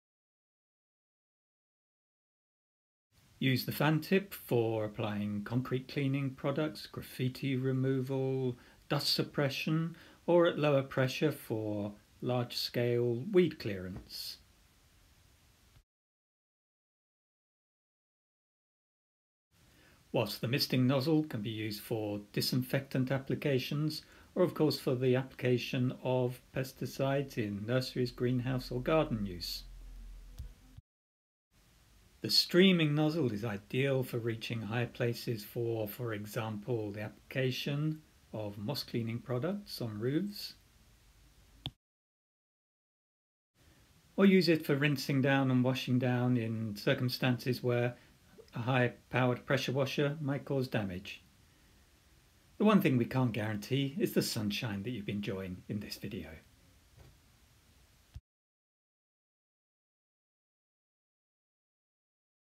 KK30 in use
See the KK30 in use with the three different nozzles